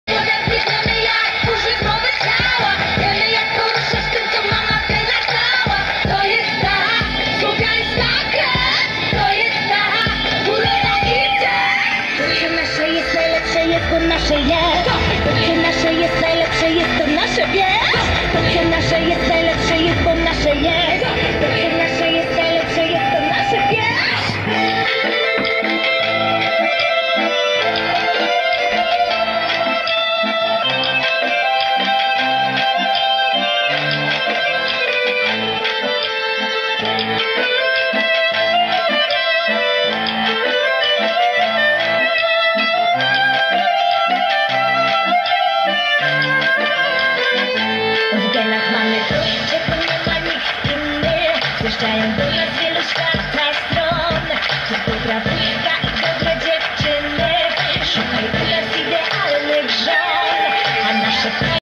Unitra GD 13-19/3W 4 Ohm
Test kolumny Od adapteru Camping.Kolumna typu Open Baffle